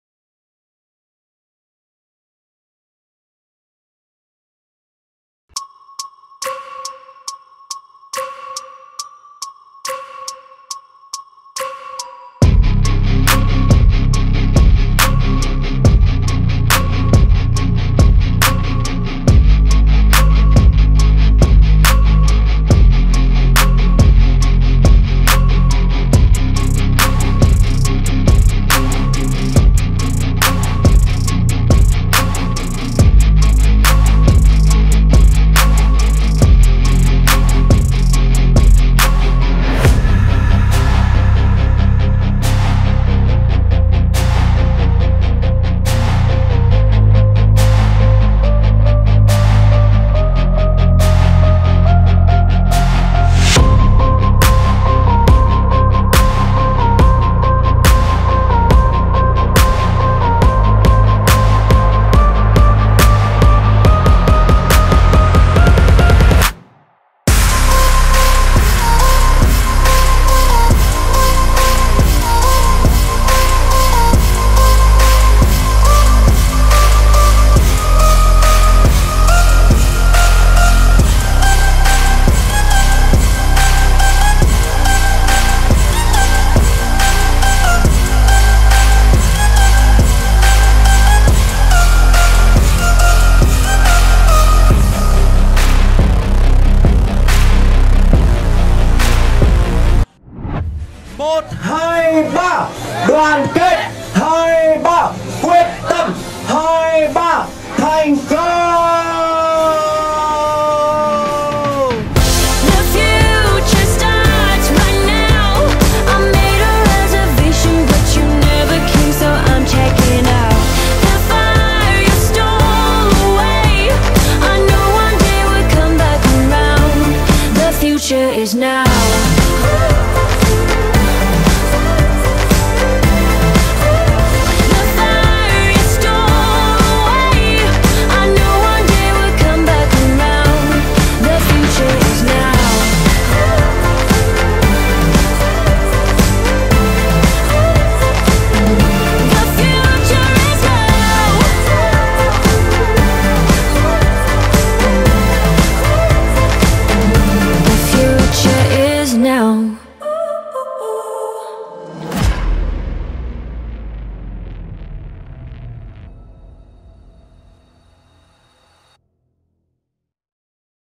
Lễ ra quân dự án The Grand Manhattan của Novaland trung tâm TP.HCM
Nhìn lại sự kiện ra quân giới thiệu dự án khu căn hộ The Grand Manhattan tại công trường dự án với chủ đề "Kim Cương Xanh" cung cấp thêm các thông tin cho hàng trăm chuyên viên tư vấn đến từ các đại lý phân phối..